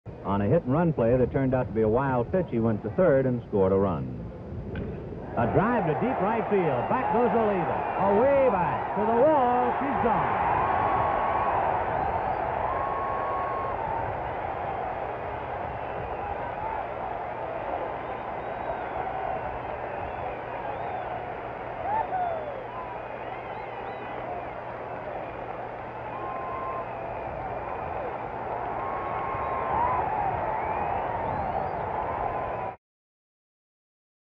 WS1965 Gm4: Parker hits solo home run for Dodgers
10/10/65: Wes Parker hits a solo home run off Mudcat Grant to extend the Dodgers' lead to 3-1 in the bottom of the 4th of Game 4